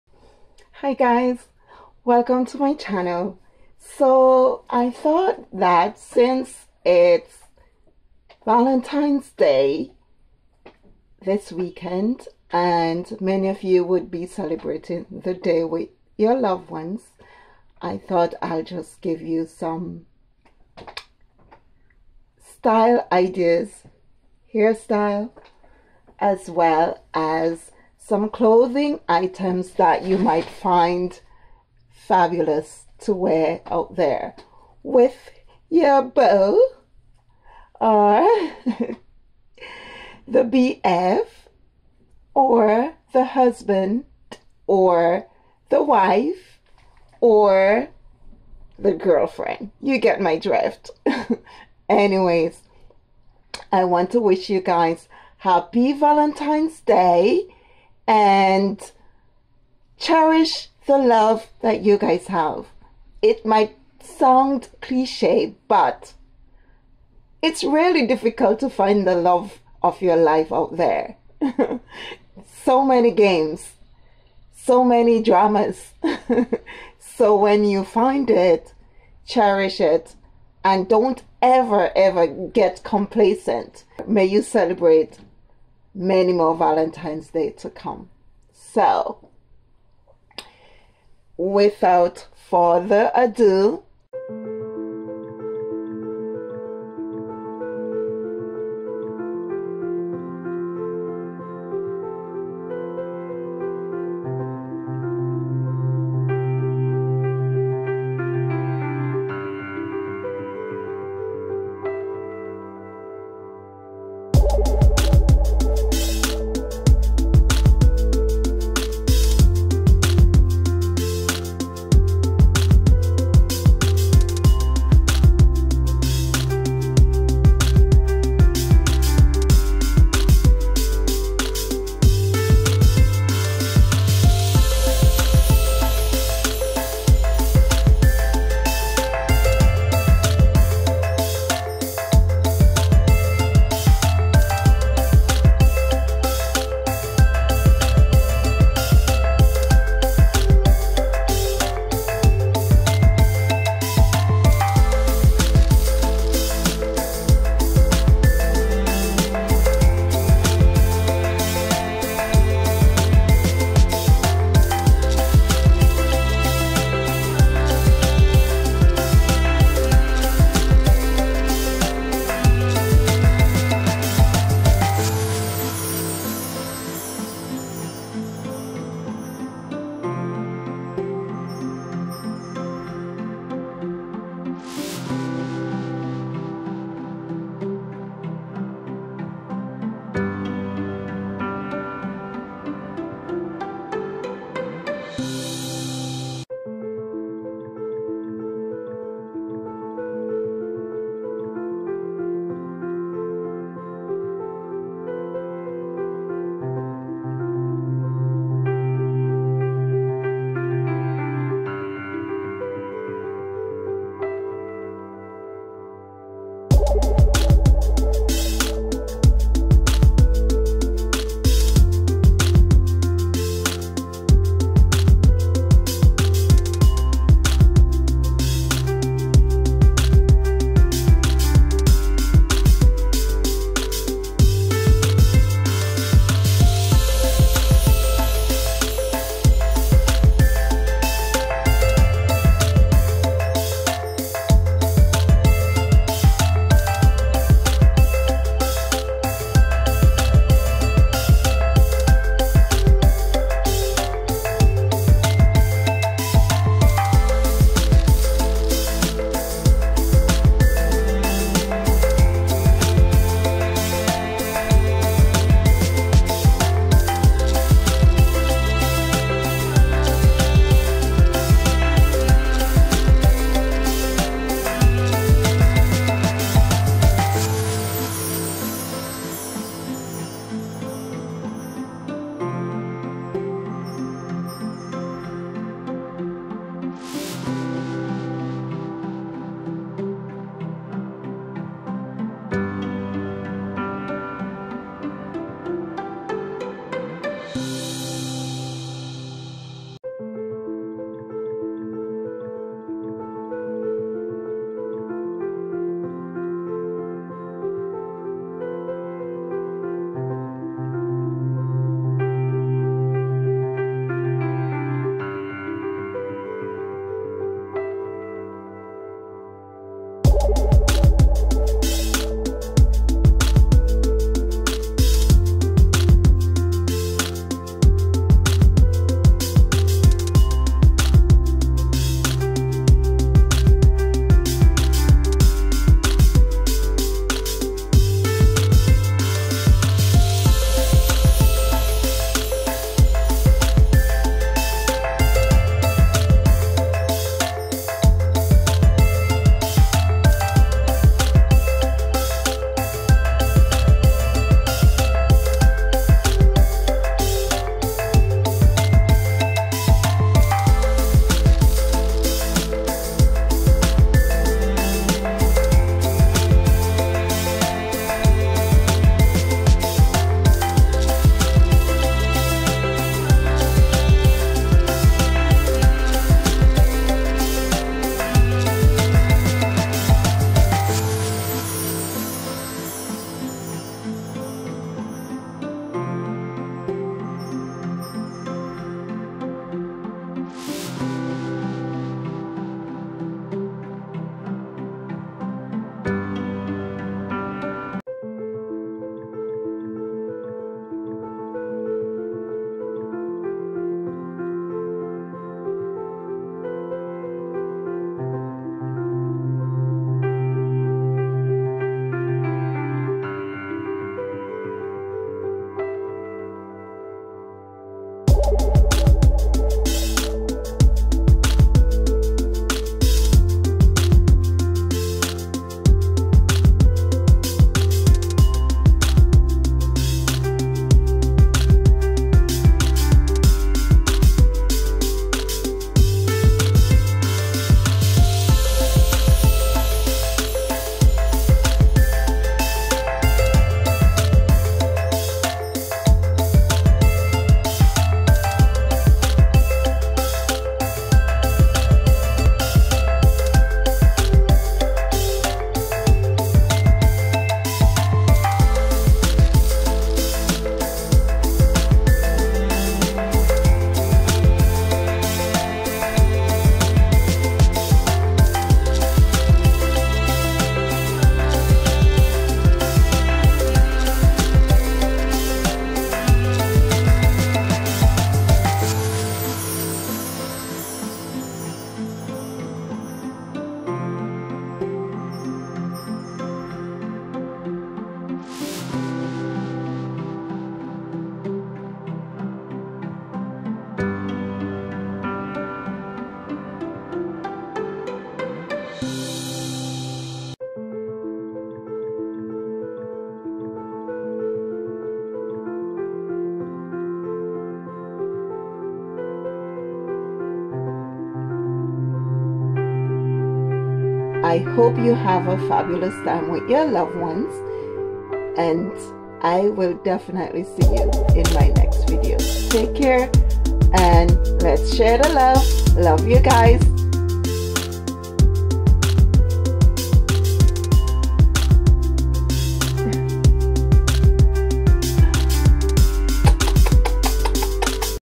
GRWM - DRESS UP FOR YOUR BAE VALENTINE'S DAY FANTASY DATE LOOK
HAPPY VALENTINE'S DAY EVERYONE!!! So, in this video I want to share a glamorous Valentine's Day look for that lucky someone who may wish to go on a fantasy date.
Dress: JJ'S HOUSE
ACCESSORIES: ALDO AND MISCELLANEOUS STORES
SANDALS: GUESS MARCIANO